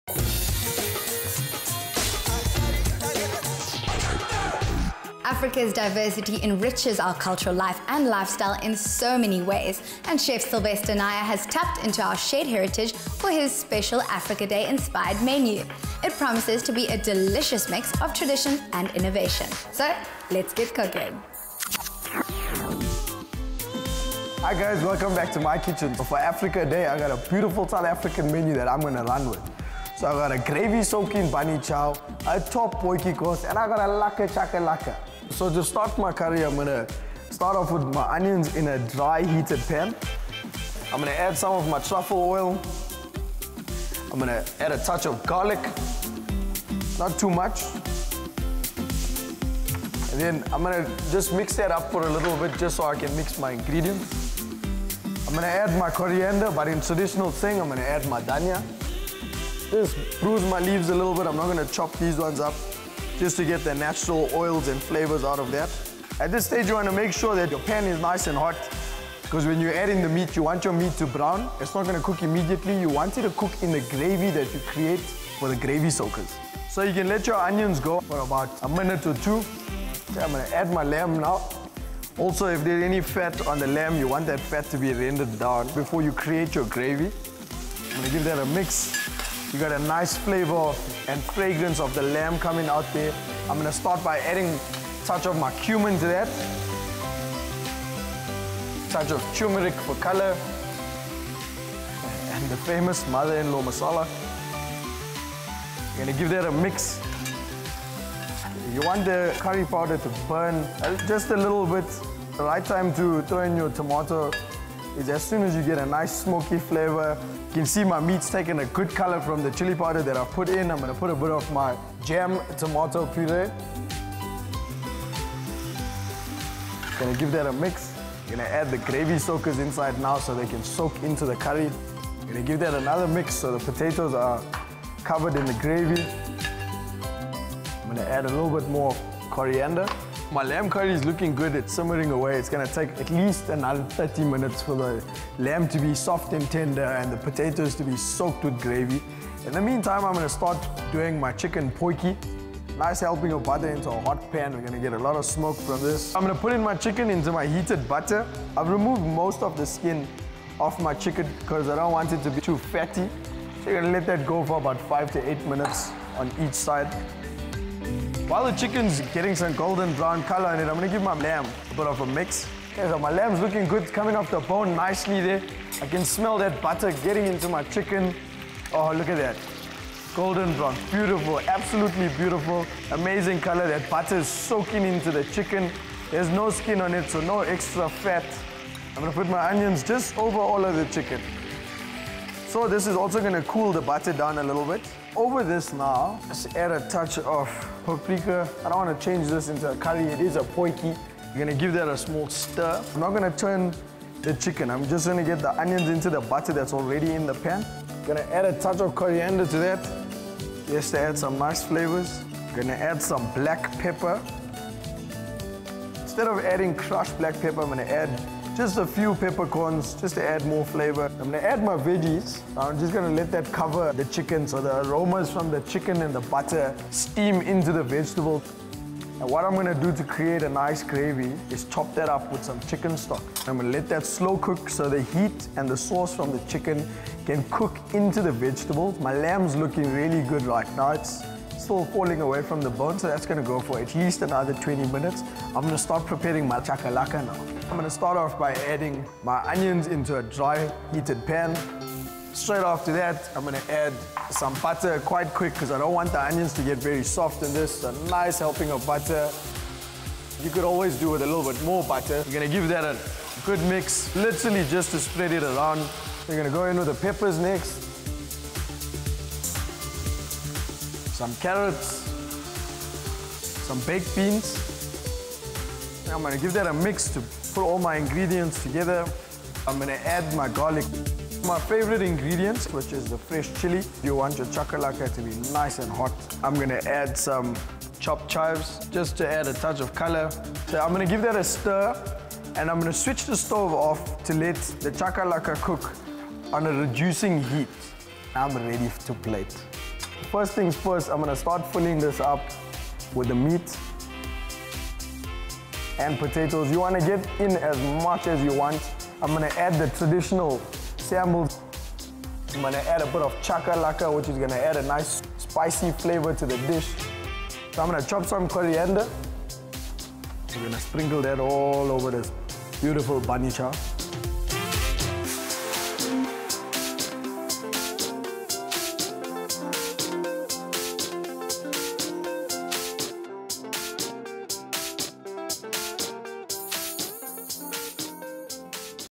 Food Mela : Sylvester Nair's Africa-Day Menu
Africa’s diversity enriches our cultural life and lifestyle in so many ways – and Chef Sylvester Nair has tapped into our shared heritage for his special Africa-Day inspired menu – with tasty lamb bunny chow, spicy chicken potjiekos and tangy chakalaka.